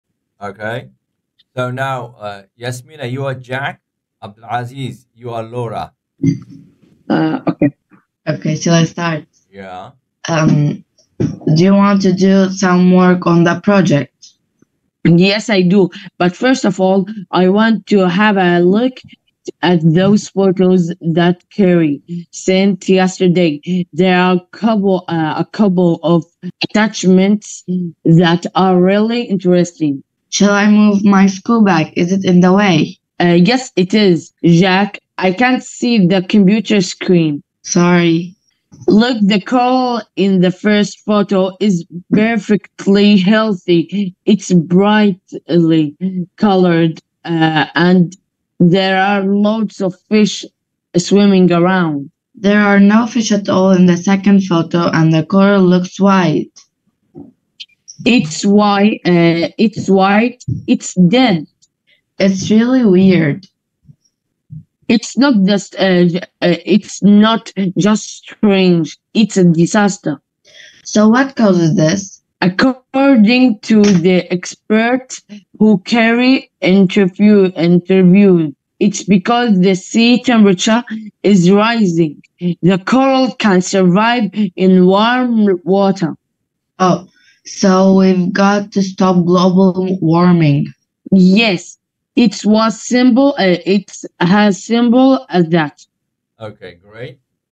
Role-play, English World 8, Reading fluency practice
English World: Role play to improve reading fluency and speaking